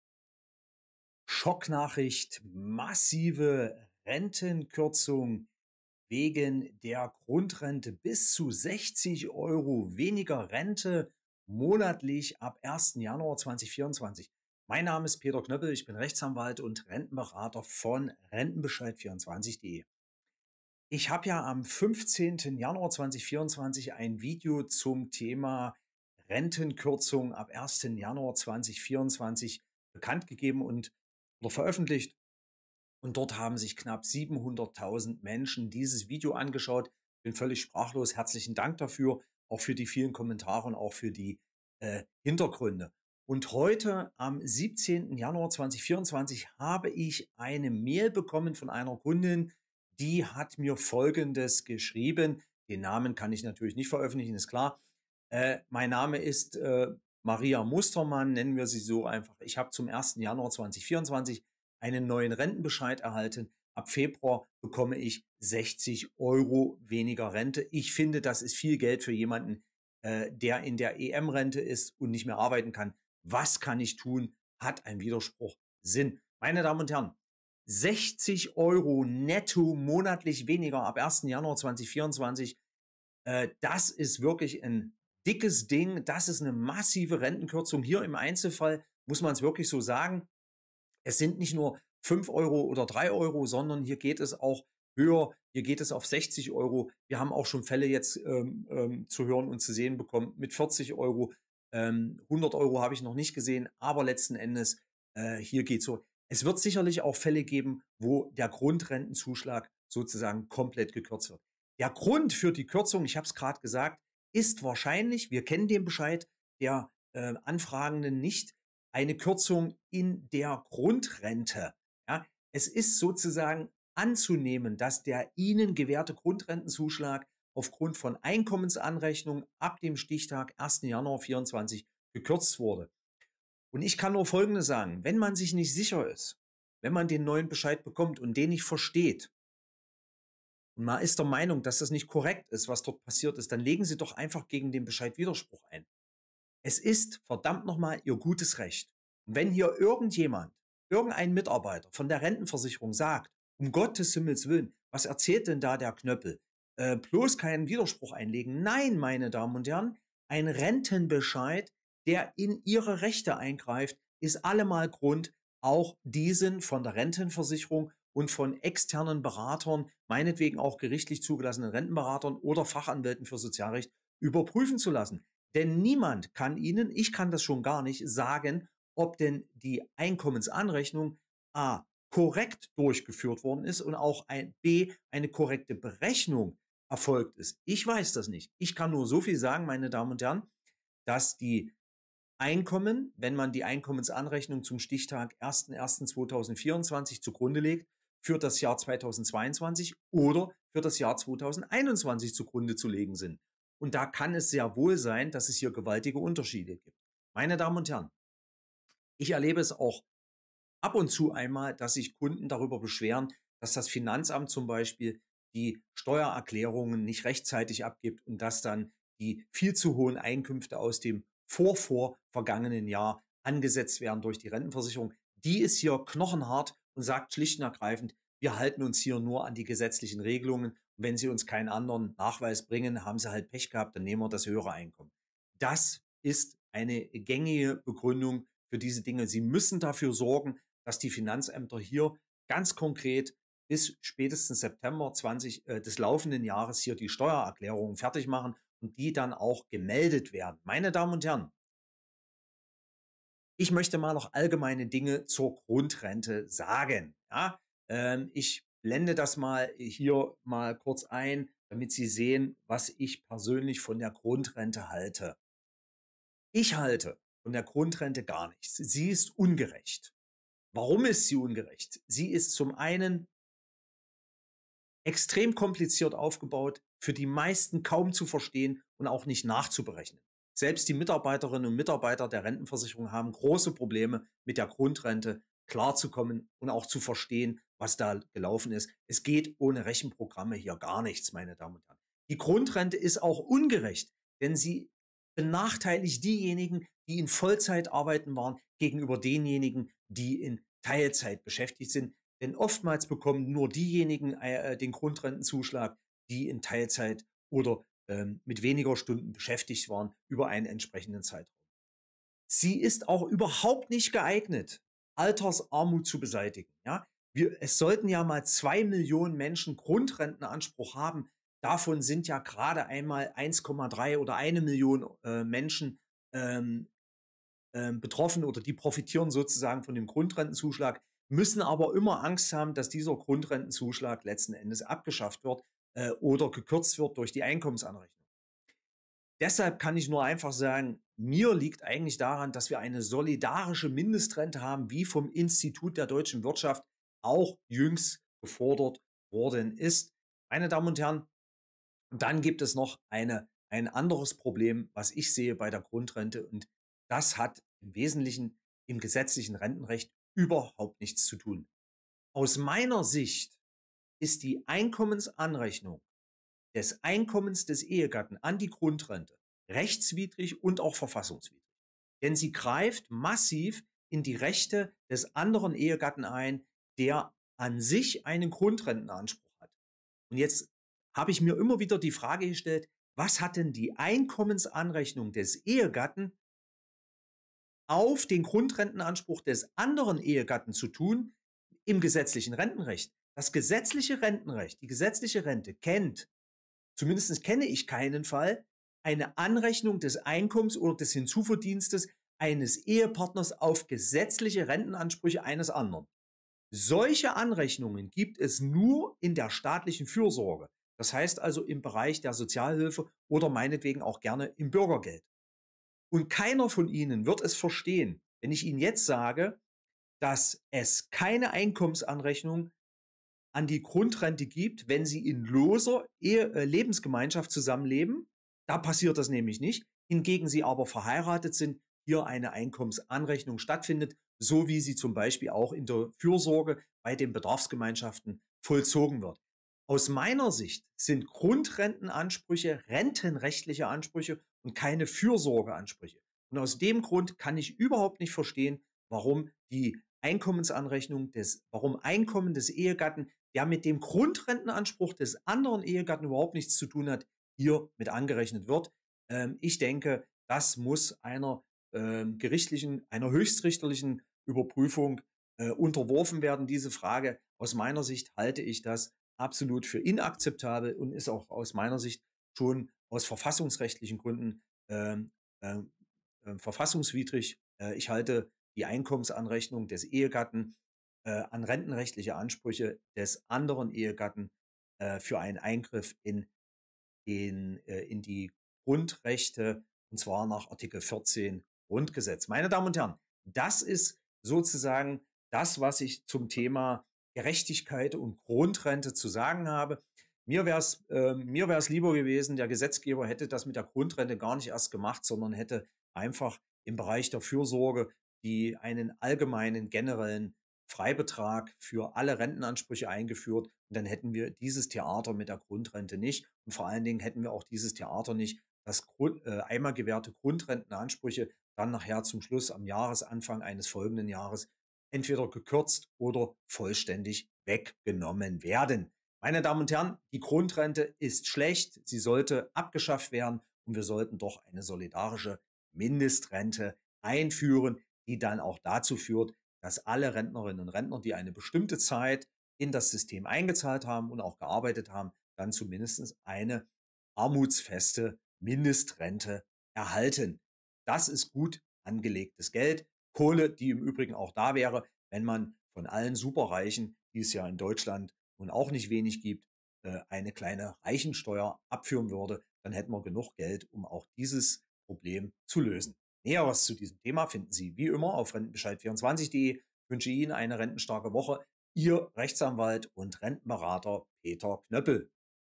Schocknachricht massive Rentenkürzung ab 2024: 60€ mtl. weniger Rente: Desaster Grundrente
Schocknachrichten: zum Teil massive Rentenkürzung wegen Grundrente ab Januar 2024.
Nicht nur das rund 1 Millionen Rentner*innen die versprochene Grundrente überhaupt nicht erhalten, es war ursprünglich die Rede von 2 Millionen Begünstigten. Nein noch viel schlimmer, viele der an sich Begünstigten erhalten Aktuell Schocknachrichten , neue Rentenbescheide kommen, Rente werden gekürzt.
Rentenberater Peter Knöppel zeigt ein Beispiel auf in dem die Betroffene ab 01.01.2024 sage und schreibe 60€ monatlich weniger Rente gezahlt bekommt, wegen der Einkommensanrechnung an die Grundrente. Wir haben hierzu schon ein Video am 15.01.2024 veröffentlicht. Weitere Informationen finden Sie in unserem Textbeitrag